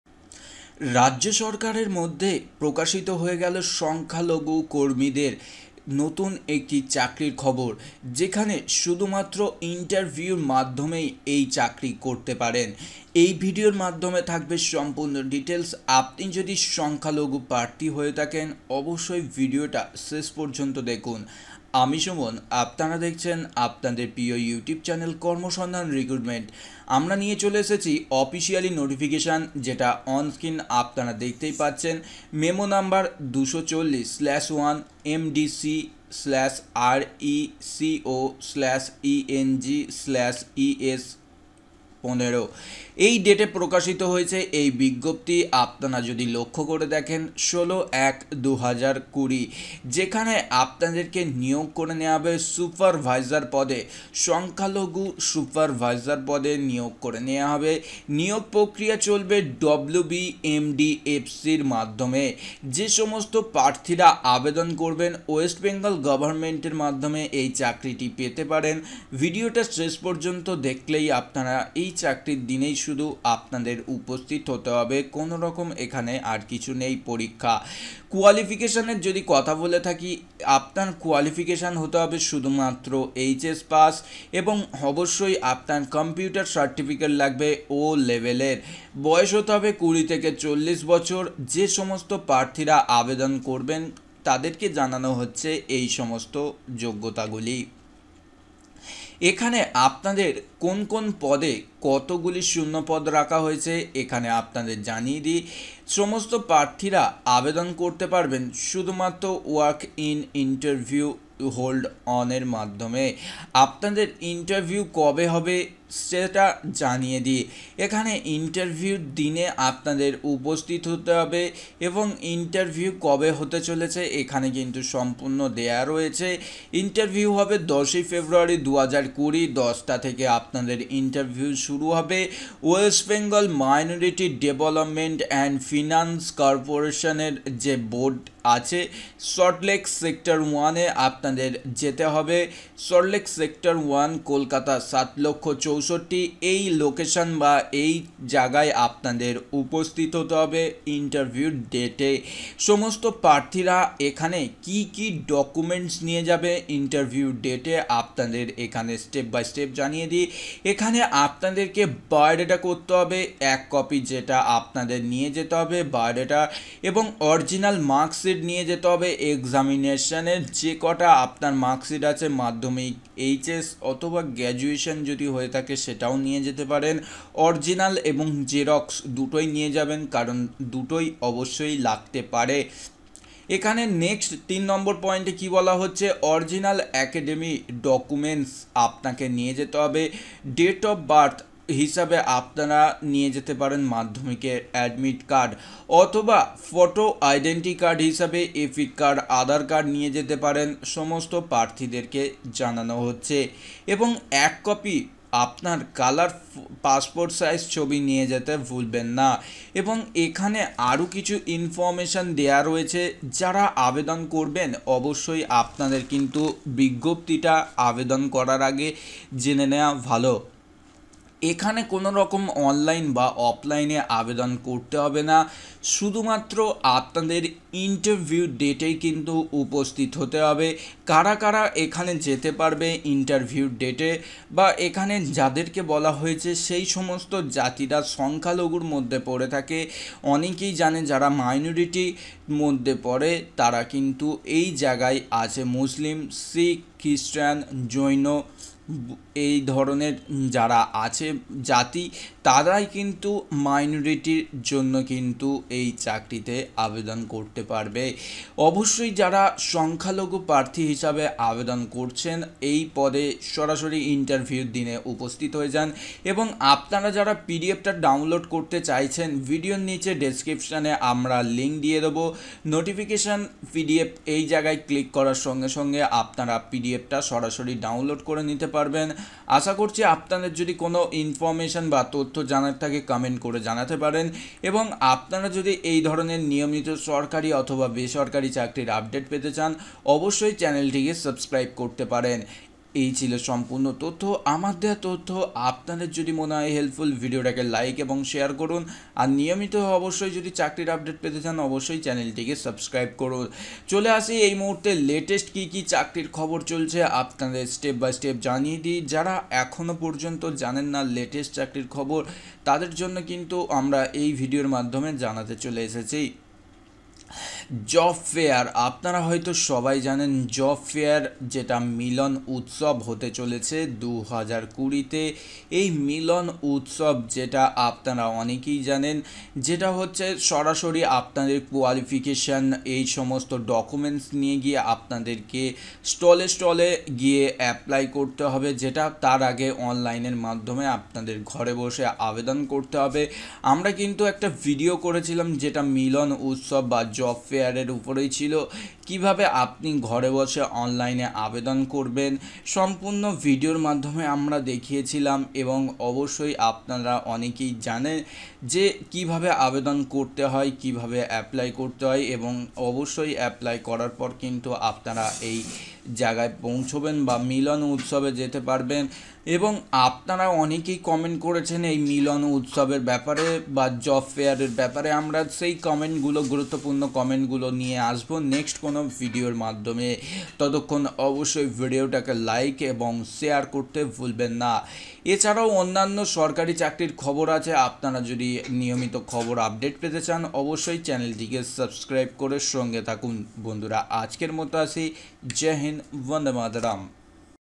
সংখ্যালঘু কর্মী নিয়োগ রাজ্য সরকারের (H.S PASS ) পরীক্ষা ছাড়াই চাকরি সুযোগ দিচ্ছে
Minority Candidates have been offered the job through direct interview of the state government with the qualification of Higher Secondary. All interested candidates are being requested to apply.